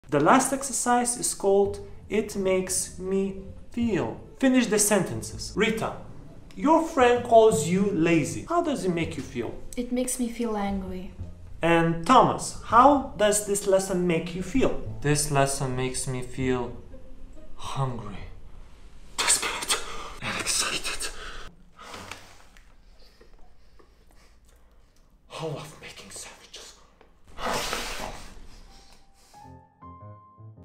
When your obsession is in the way with the lesson subject. Learn English with fun
You can support us and make us grow by buying us a cup of tea/coffee

Video directed by Justas Ivanauskas
Actors: Justas Ivanauskas, Elzė Bog
Videographer: Mindaugas Balkevičius
Video Editor: Vėtris Butkus

Video description. It's an English lesson. Teacher Justas tries to help students Rita and Thomas find out how to use phrases with Make. Events take a turn when the main objective of the lesson for some becomes making (and eating!) sandwiches.